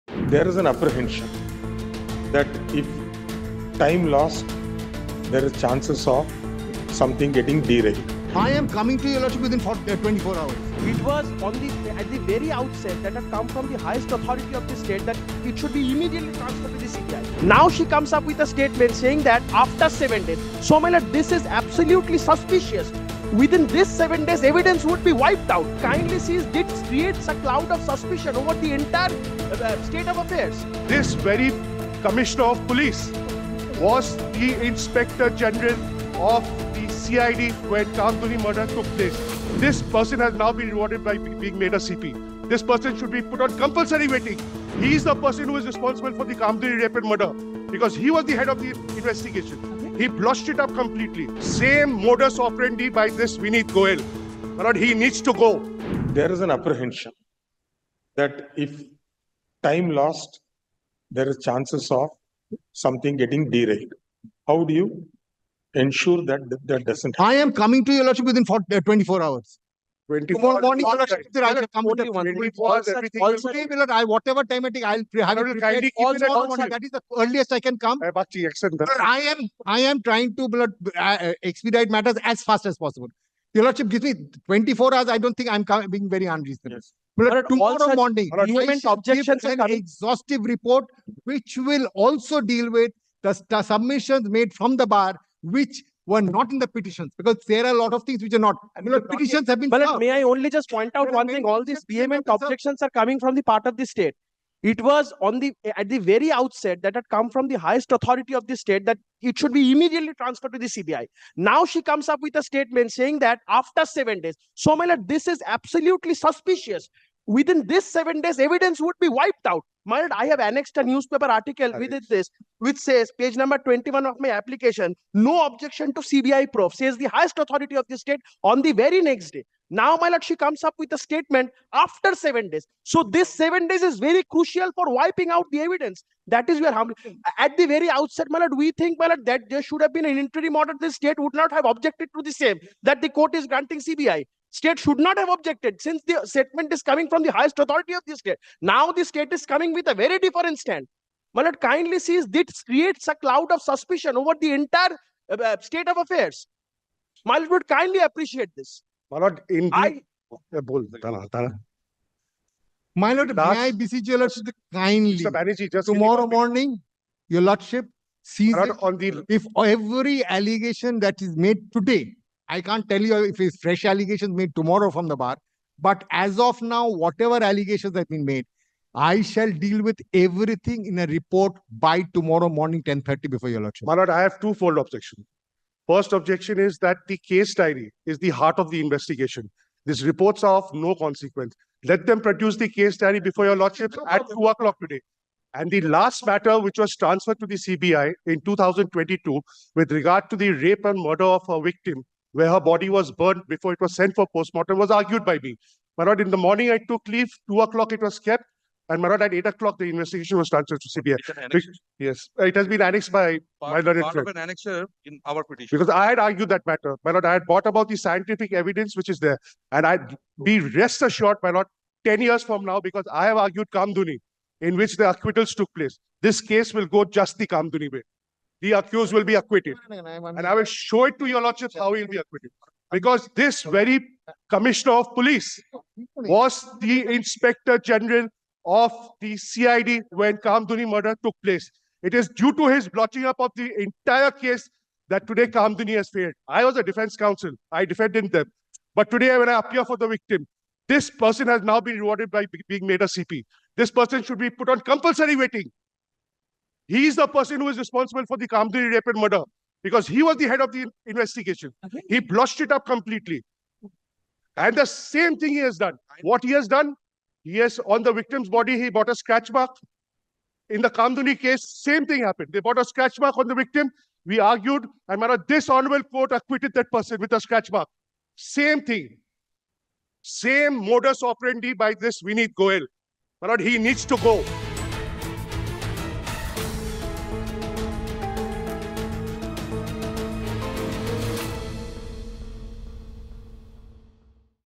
‘Evidence will be wiped out…’: Heated exchange in High Court over Kolkata doctor rape-murder case
Heated exchange took place in the Culcutta High Court on August 13 over Kolkata rape-murder case of trainee doctor of RG Kar Medical college and Hospital.